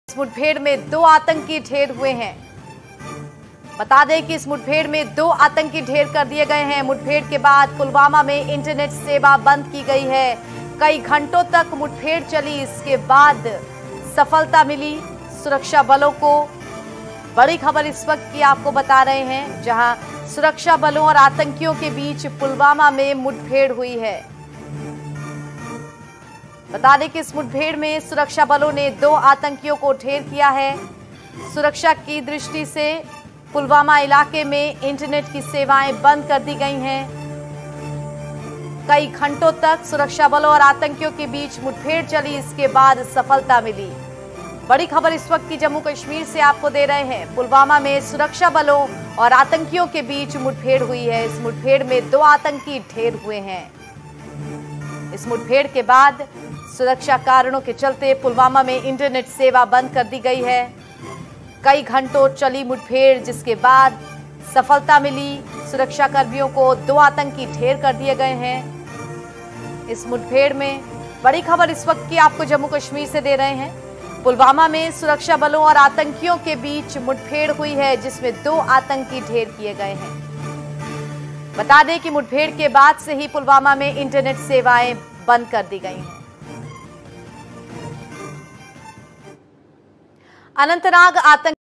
Jammu - Kashmir : पुलवामा में सुरक्षाबलों और आतंकियों में मुठभेड़ || BREAKING NEWS || National Voice
About Channel